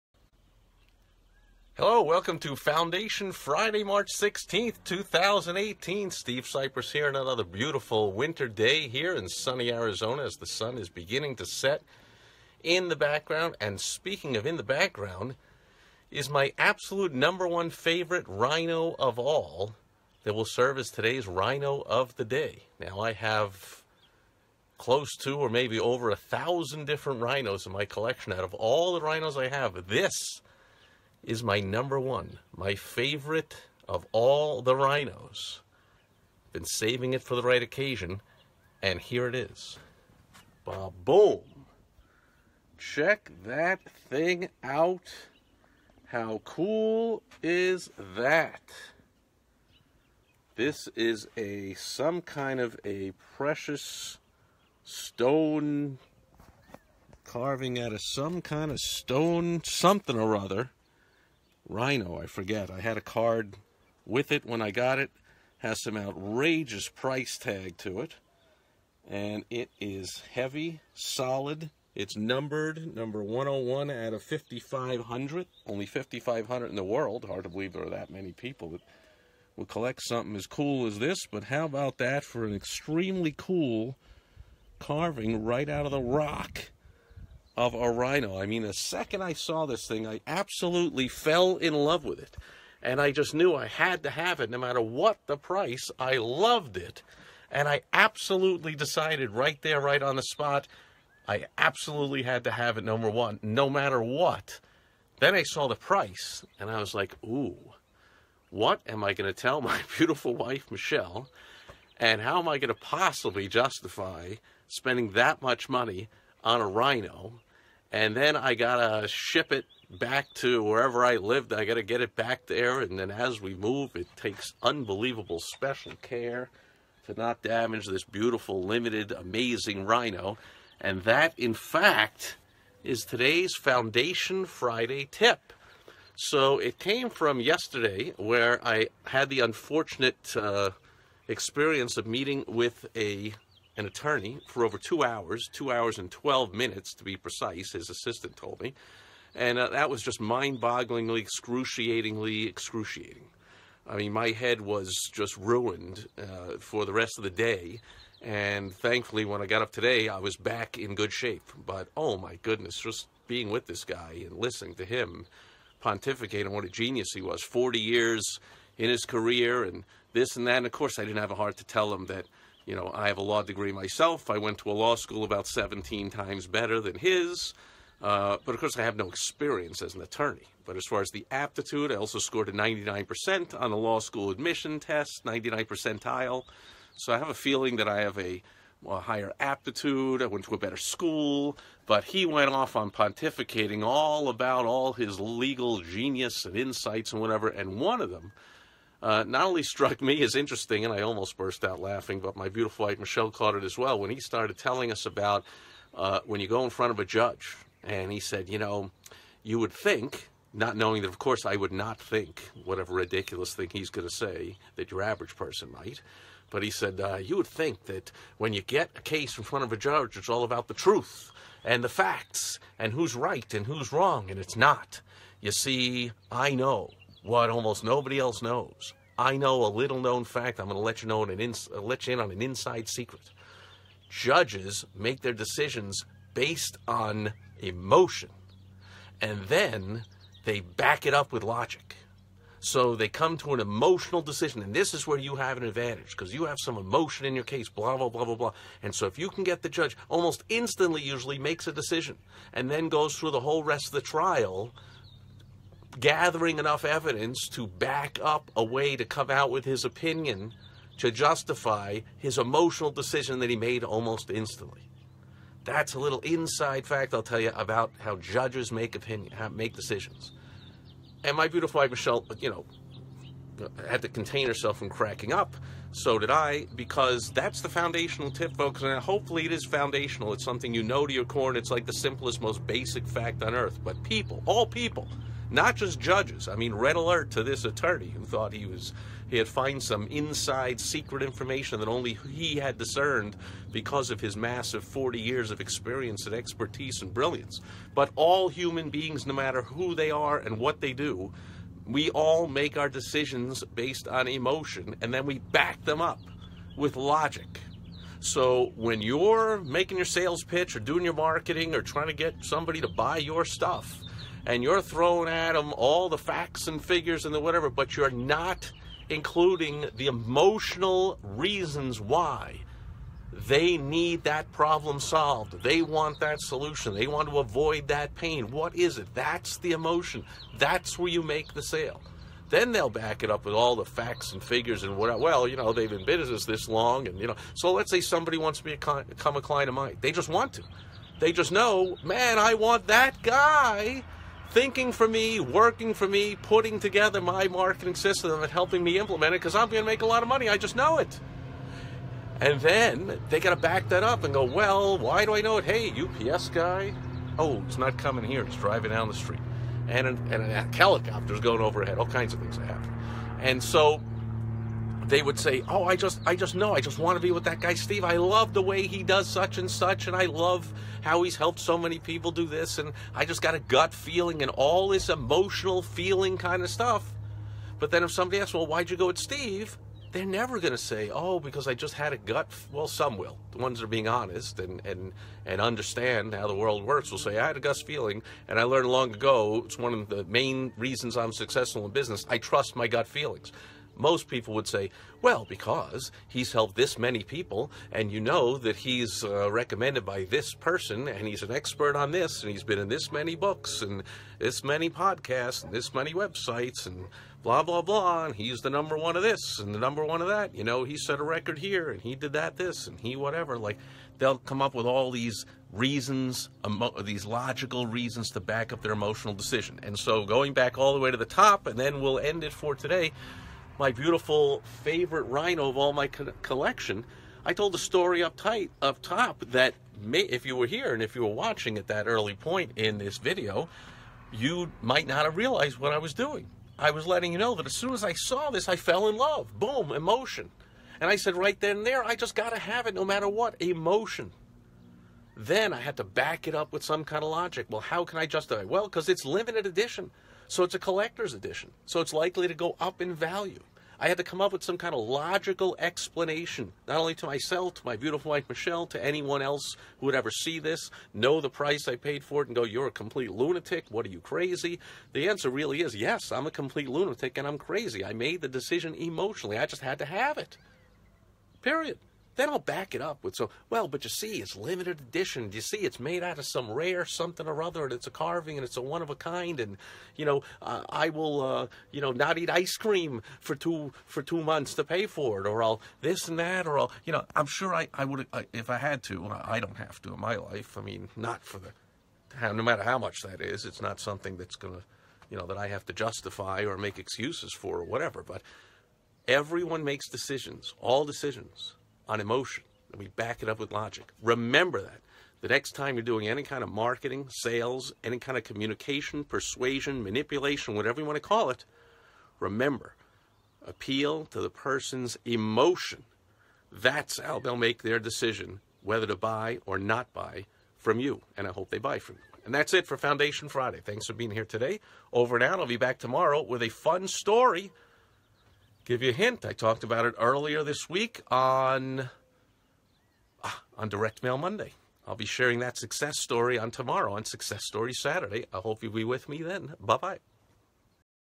Foundation Friday - How People Really Make Decisions
An attorney I met with yesterday told me a mind-boggling fact that he discovered.

(*Plus video of my #1 most favorite rhino of my collection of over a thousand of them.)